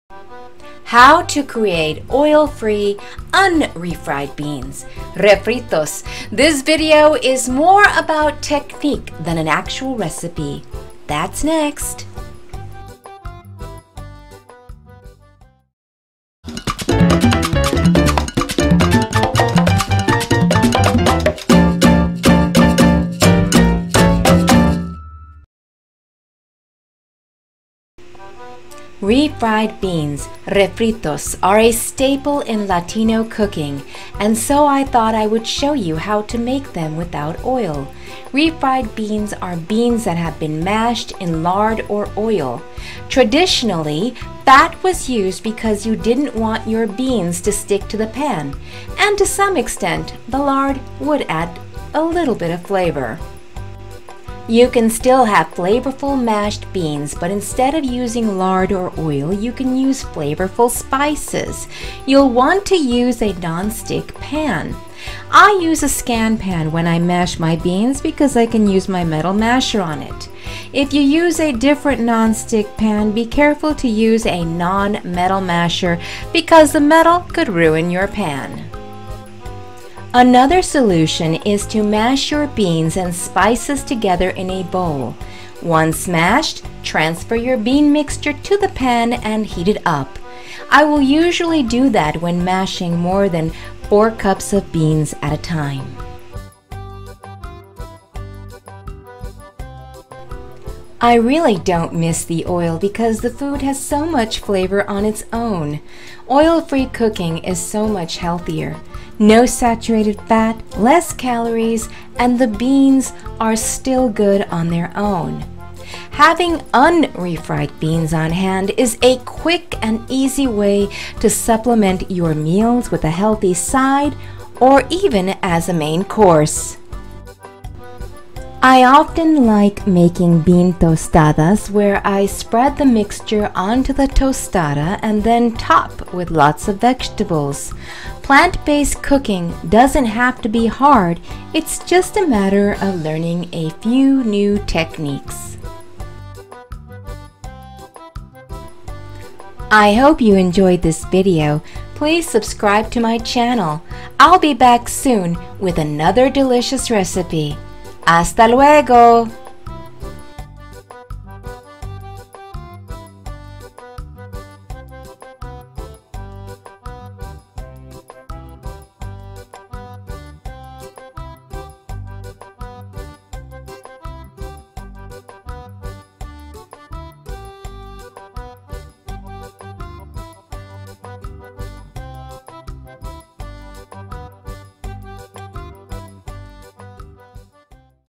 Whole Food Plant Based Oil Free Refried Beans REFRITOS Vegan WFPB No Oil Vegan Fat Free Easy Recipe
Today I'm going to show you how to cook oil free refried beans "Refritos". This is a an easy Latino dish that is a staple with most meals. I also included a quick fix meal using refried beans.

WFPB Oil Free Refried Beans:

Ingredients:

2 cups of Beans
1 tbsp Mexican Seasoning
1/4 to 1/2 cup of Liquid

Liquid - Water, Broth or Bean Liquid

Mexican Seasoning - I recommend getting a Mexican seasoning to make your life easier or you can make your own mix.

Easy Mexican Seasoning:
1/2 tsp Ground Cumin
1 tsp Mexican Oregano
1/2 tsp Smoked Paprika
1/2 tsp Onion Powder
1/2 tsp Garlic Powder
Chili Powder to taste (optional on whether you want it spicy)

 Directions:

Mash the beans and liquid together. If you have a scanpan than you can do it directly on the pan. If you don't have a scanpan then carefully use a silicon or plastic masher. You can also mash your beans beforehand and then cook the mixture til it thickens up. Use extra liquid as needed.

I prefer to use a masher and not a food processor or blender. Refried beans are mashed so that you have a little texture. If you put them in a food process or blender, they become pureed beans and you will miss out on the texture of mashed beans which is one of the best parts about the dish to begin with.

Quix fix meal idea with Oil Free Beans;

Ingredients:

Oil Free Tostadas
Refried Beans
Veggies

Directions:

Spread the refried bean mixture onto the tostadas. Top with vegetables, salsa, guacamole, vegan crema and more...